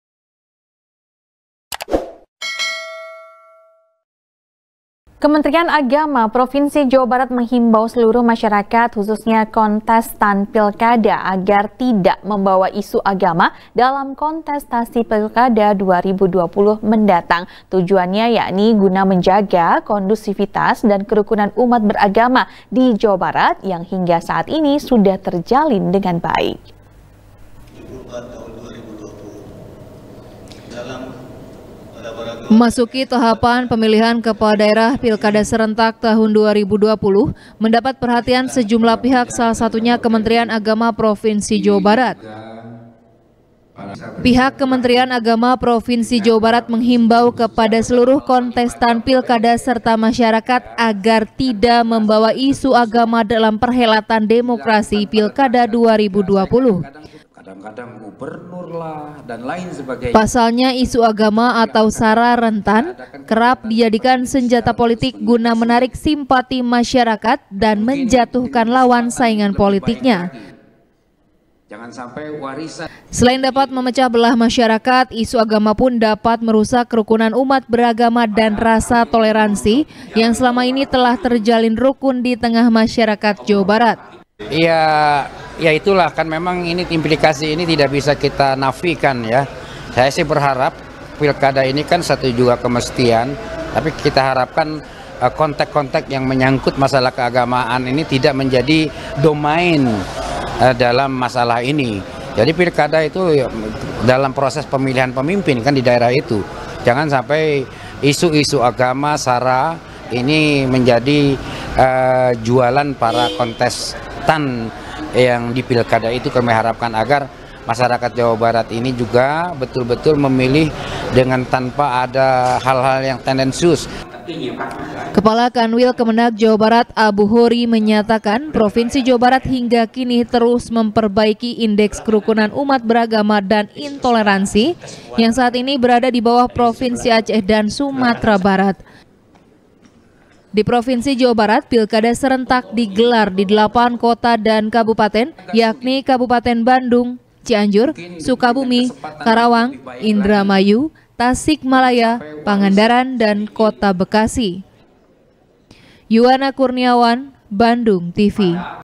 KEMENAG Himbau Jangan Bawa Isu Agama Dalam Pilkada 2020 | SBR | BANDUNGTV
SBR | BANDUNGTV | KRISANTY | Kementrian Agama Provinsi Jawa Barat Menghimbau Seluruh Masyarakat Khususnya Kontestan Pilkada,Agar Tidak Membawa Isu Agama,Dalam Kontestasi Pilkada 2020 Mendatang.Tujuannya Yakni Guna Menjaga Kondusifitas Dan Kerukunan Umat Beragama Di Jawa Barat,Yang Hingga Saat Ini Sudah Terjalin Dengan Baik.